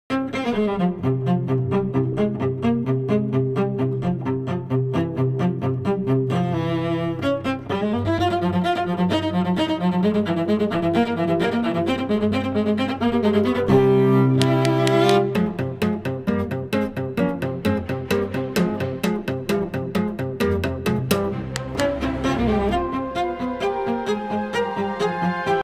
Wednesday Addams Artwork 😍
Watercolor painting time-lapse of Wednesday Addams.